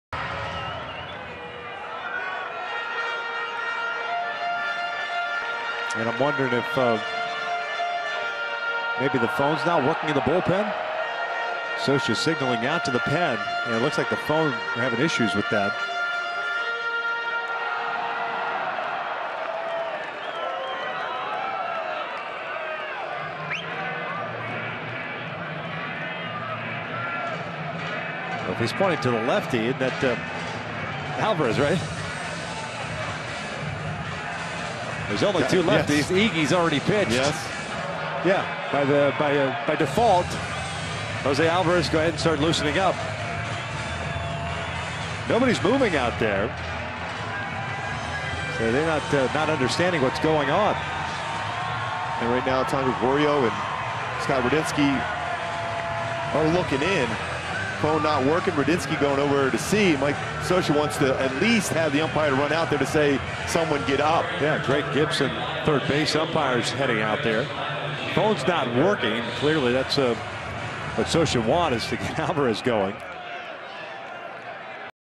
LAA@TEX: Scioscia, bullpen confused after phone fails
Mike Scioscia attempts to contact the bullpen, but due to the dugout phone not working there is a lapse in communication